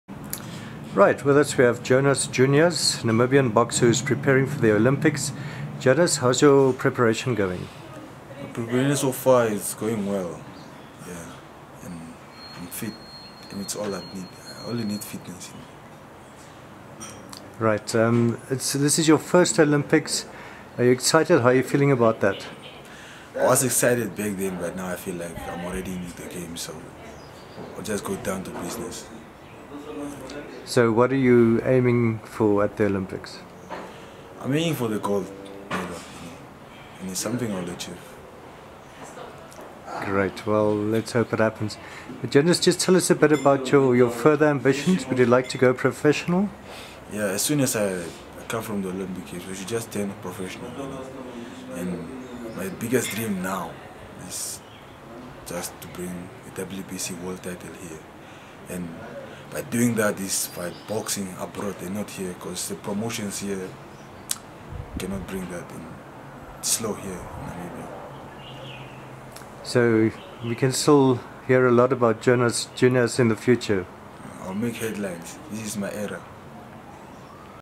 Jonas Junias
Namibian boxer Jonas Junias talks about his preparations for the Rio Olympic Games.